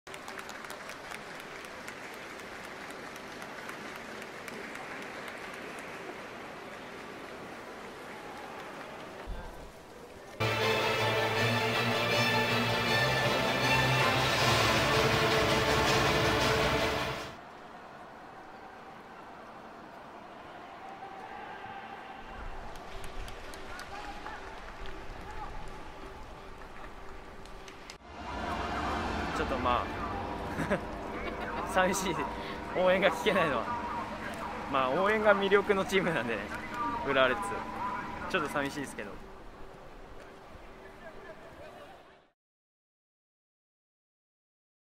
Japanese football fans watch match in silence under new COVID rules
The rules were in place for the 7000 fans at the 2002 Saitama stadium.